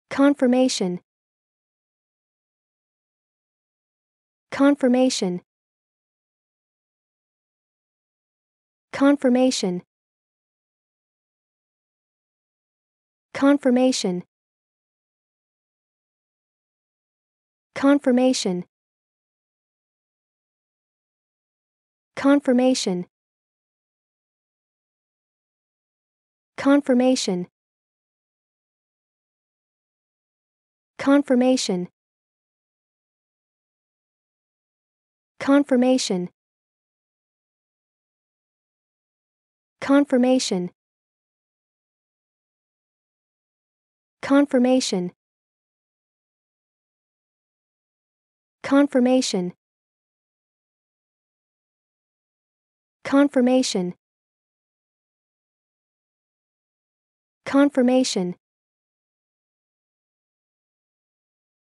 Como dizer 'confirmação' em ingles?
Como dizer confirmação em Ingles? Como se pronuncia confirmation? How to pronounce confirmation?